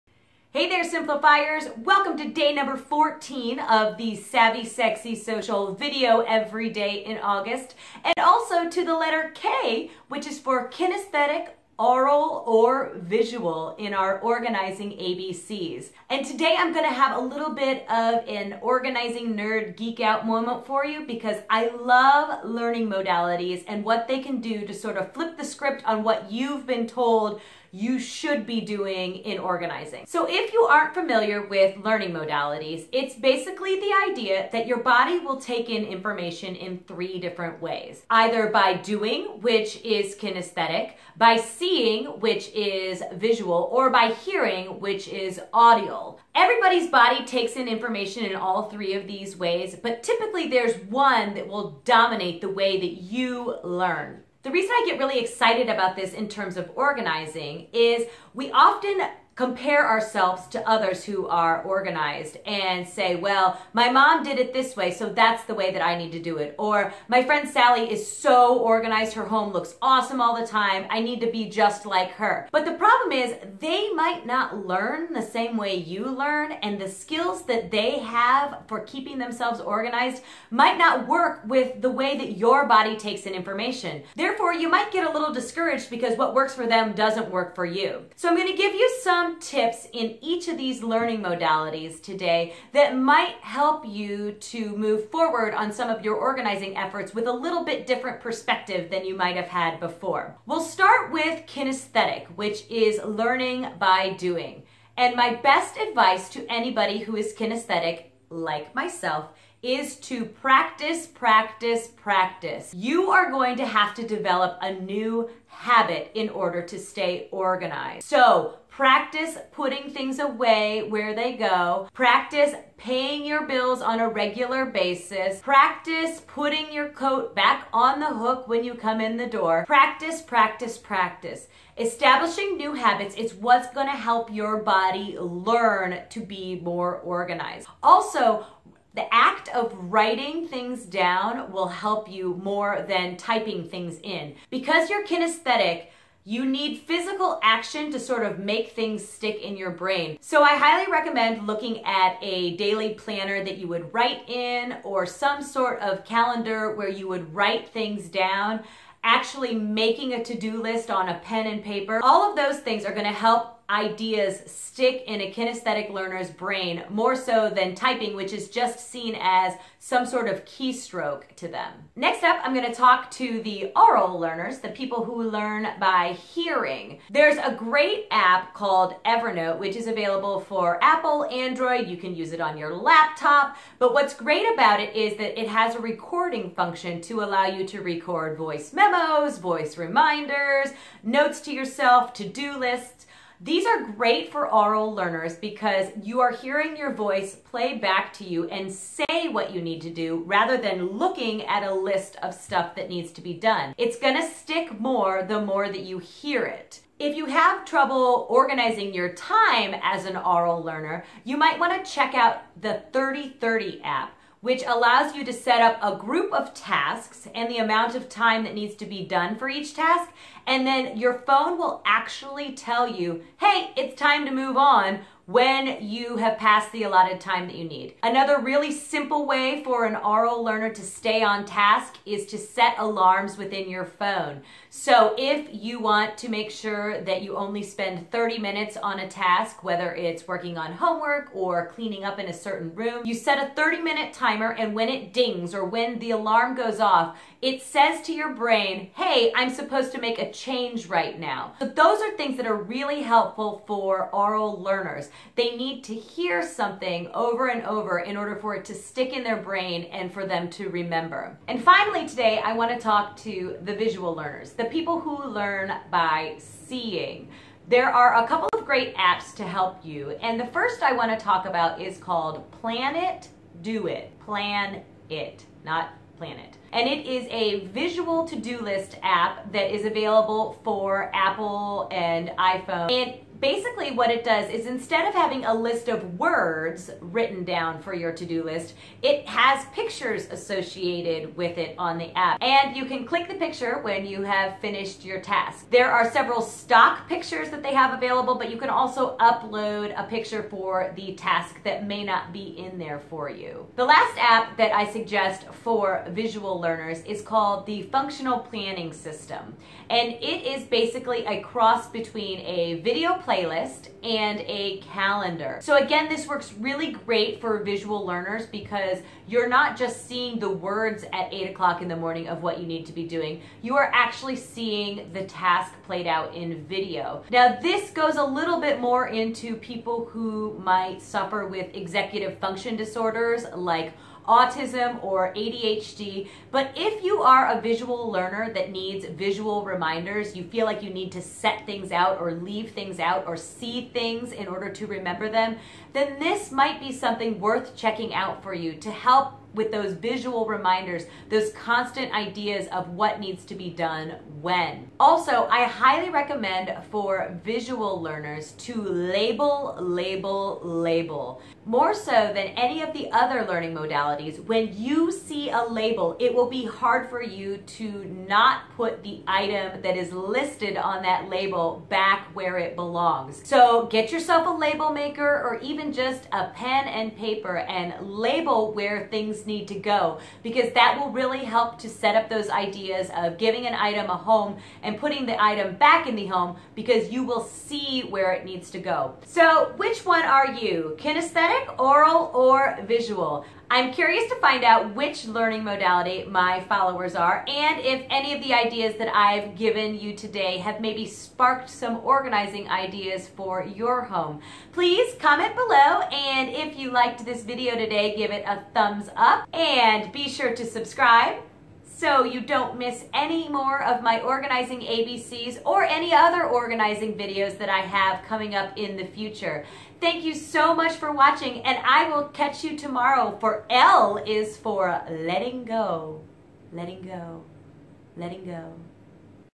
Organizing ABC's - K is for Kinesthetic, Aural or Visual
Total organizing nerd alert!  I am always looking for outside the norm ways to help my clients understand and implement organizing habits into their daily lives.  Today, I am discussing how your learning modality (either kinesthetic, aural or visual) may help you figure out organizing methods that will work better for you.

The 3 modalities I discuss today are learning by doing (kinesthetic), learning by hearing (aural) or learning by seeing (visual).  I offer some organizing strategies in each modality.

Be sure to stop back by tomorrow for L is for Letting Go!